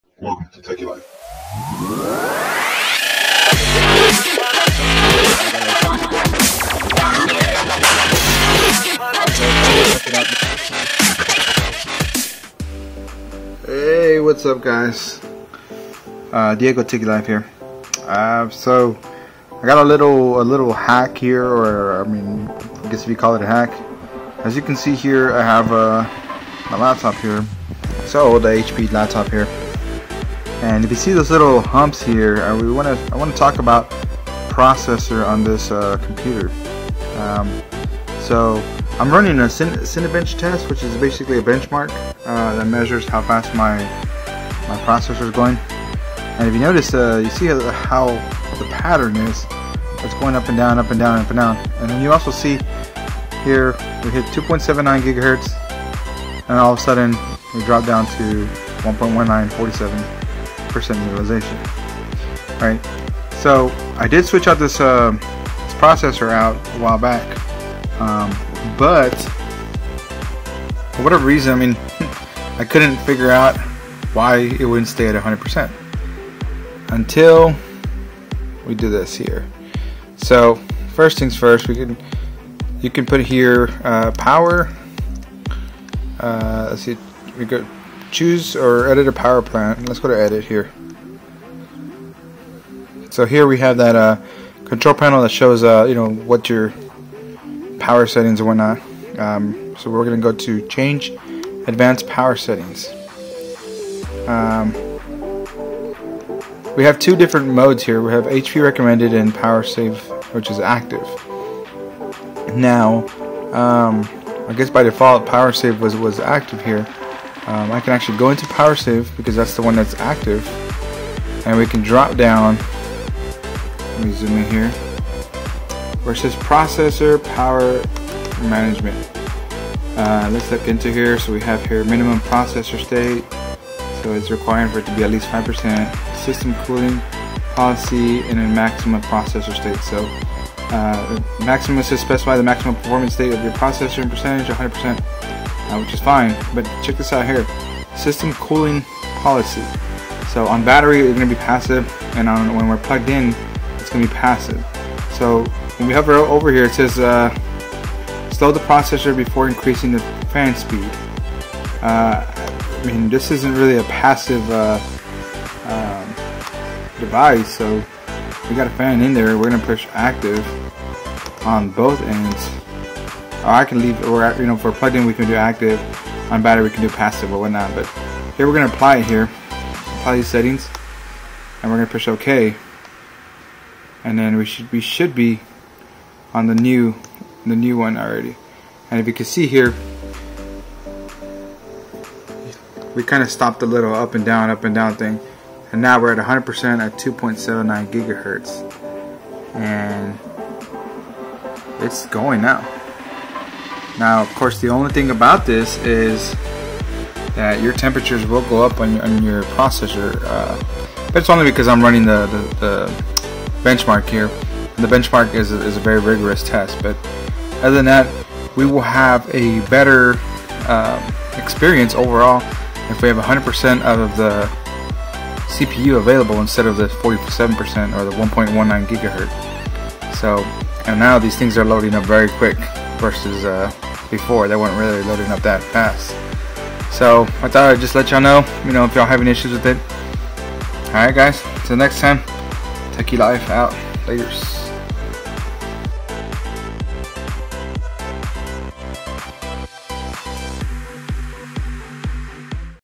Slow laptop computer fix for windows 10 | Easy 🤯🤯
Here's a simple fix, or hack that you can do if your computer is acting up or is acting slow.
This won't work will all computers but it's worth a shot to look at.